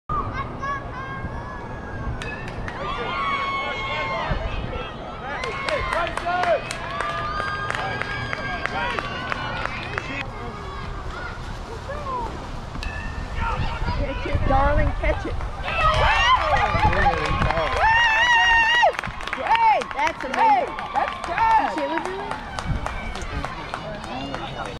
Grayson Bunch Two Big Catches 8U Sulphur Springs Tournament - Paris Haymakers 2021
Grayson Bunch makes two huge catches against the Stickmen Shock and the Brigade 8u Teams in Sulphur Springs Texas.